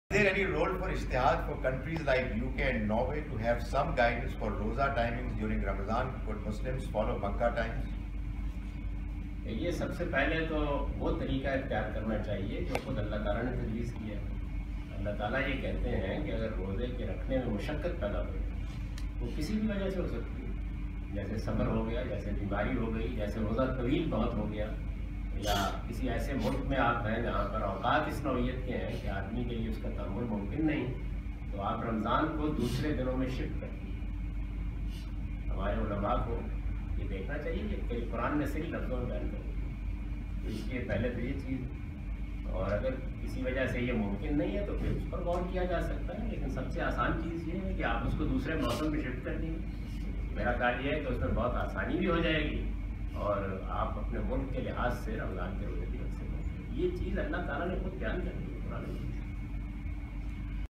Fasting in Scandinavian Countries | Javed Ahmad Ghamidi
javed ahmad ghamidi, Fasting in northern countries where long days are there? what is the option? For More Information please visit